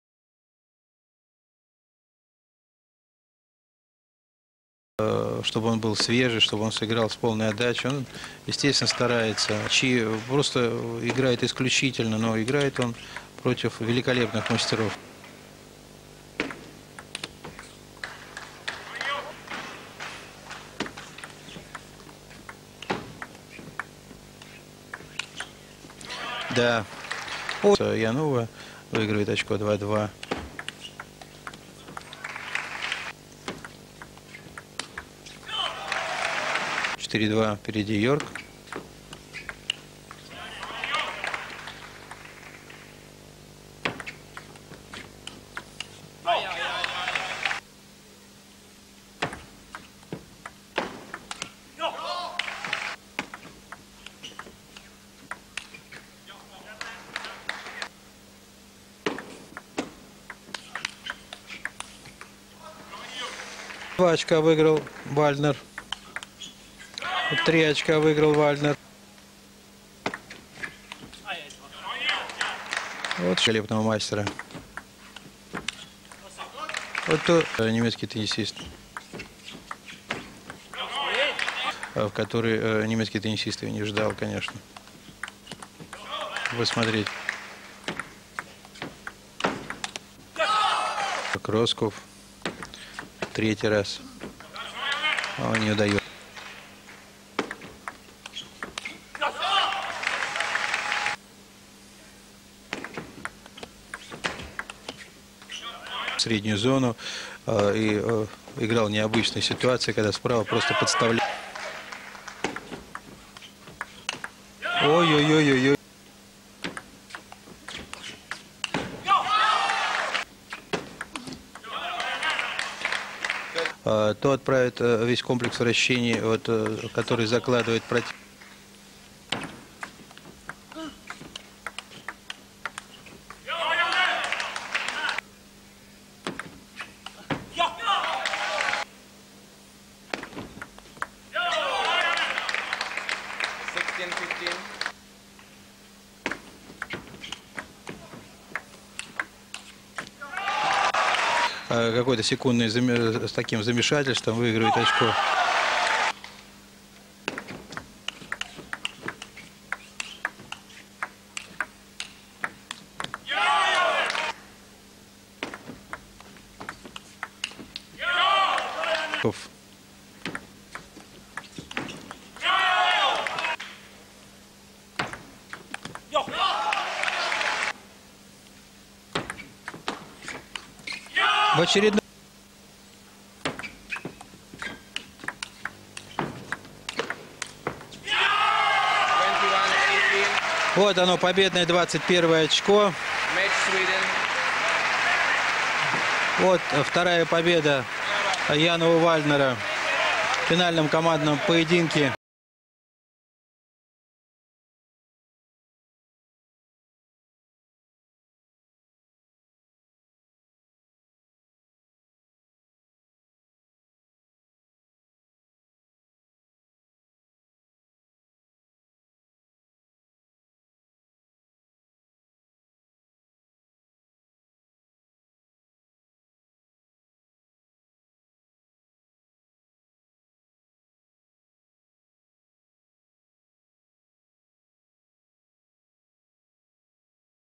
JAN-OVE WALDNER vs JORG ROSSKOPF (European table tennis championship 2000, team final)
Чтобы получить бесплатный закрытый 90-минутный видео-курс "Секреты техники для любителей" перейдите по этой ссылке
European Table Tennis Championship 2000 (Bremen)
Men’s team final Germany vs Sweden
Jan-Ove Waldner vs Jorg Rosskopf

It was one of last titles of Swedish national team at European table tennis championships.

Sweden won 4:1.

Here you can see very good match of table tennis legend Jan-Ove Waldner. As you know he won table tennis Olympic Games 1992
Final Jan-Ove Waldner – Jean-Philippe Gatien 3:0

At ETTC 2000 (European table tennis championship) Jan-Ove Waldner took gold medal in team and bronze medal in singles. He lost Zoran Primorac at semifinal 2:3.

- jan-ove waldner, jorg rosskopf, table tennis best